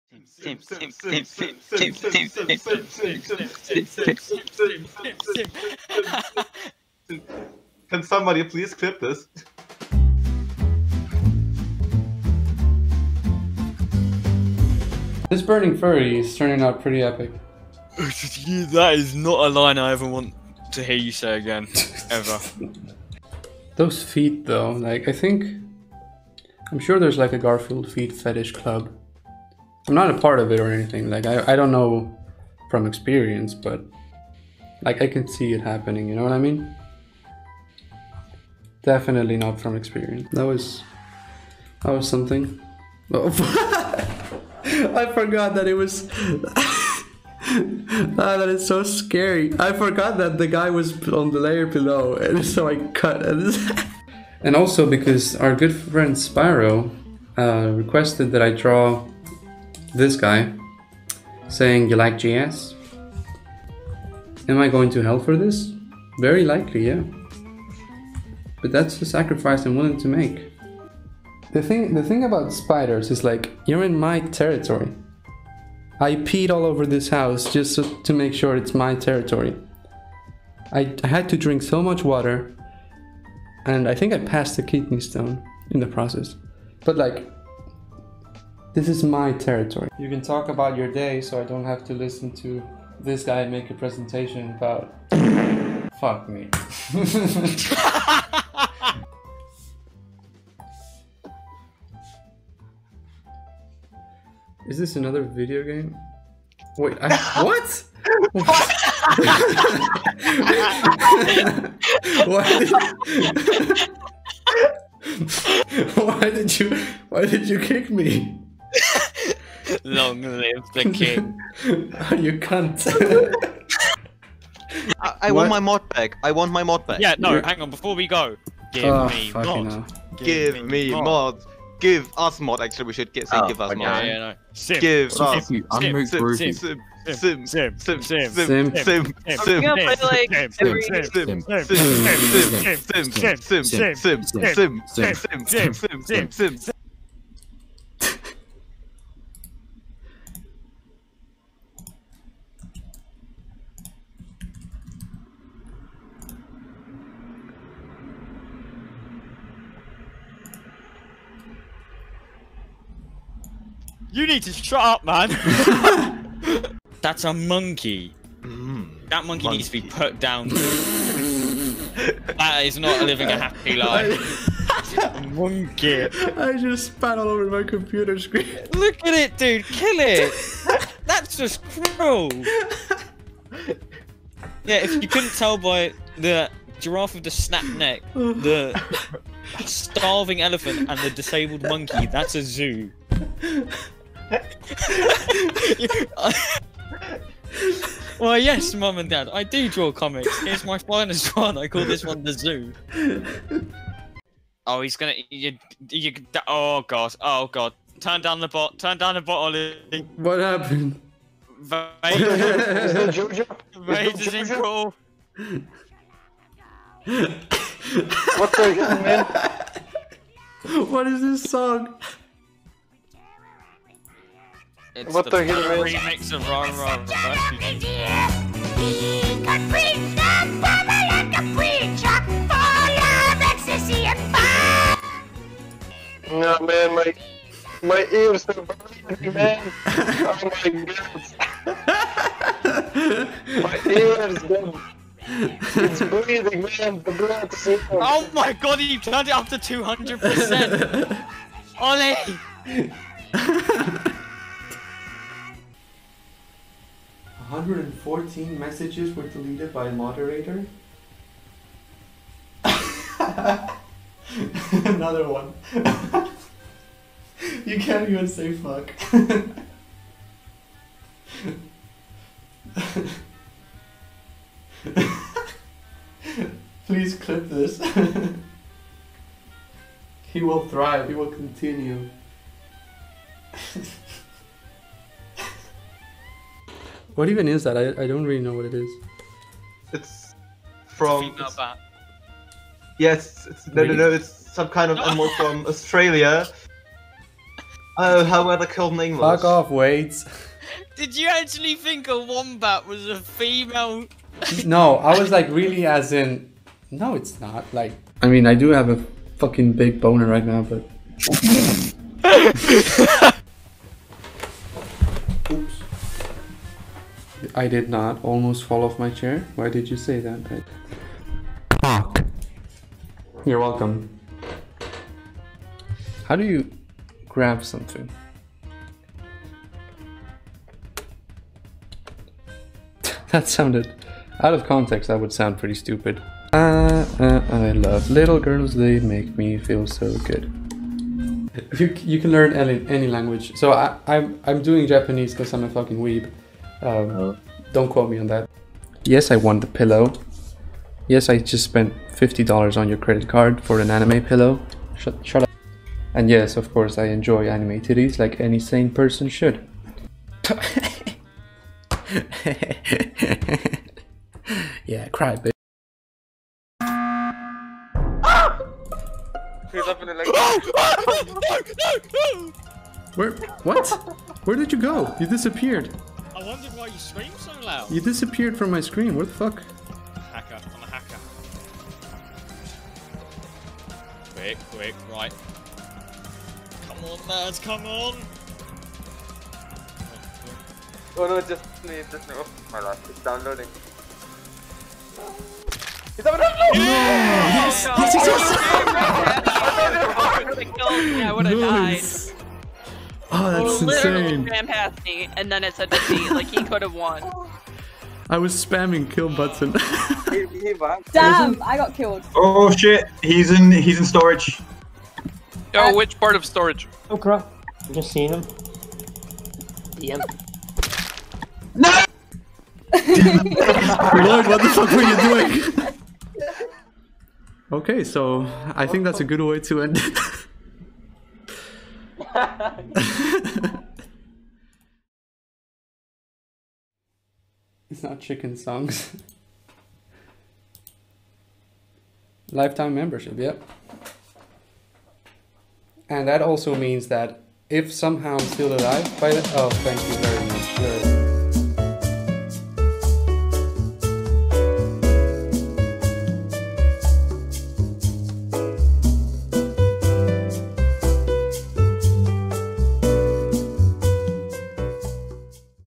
2020 STREAM HIGHLIGHTS - My Favorite Twitch Clips!
2020 was a pretty bad year already, but I thought I would make it even worse by streaming my comics on Twitch! Here are some of my favorite moments from this dumpster fire of an idea :-)

Other socials!